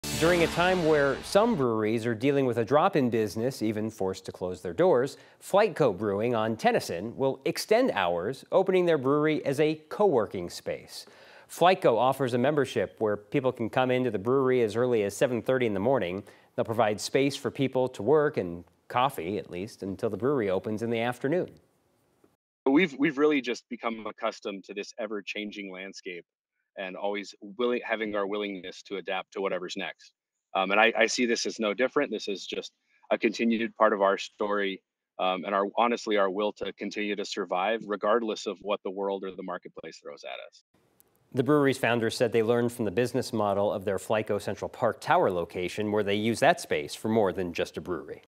Denver brewery opens coworking space
FlyteCo Brewing on Tennyson is extending its hours, providing work spaces and coffee starting at 7:30 a.m.

9NEWS (KUSA) is located in Denver, Colorado.